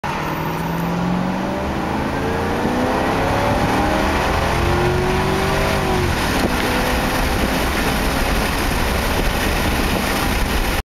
1998 Lincoln Mark VIII 0-70ish Stock
My new 98 Mark VIII I just got. 0-70ish on a 85 degree day. I plan on doing some mods in the near future.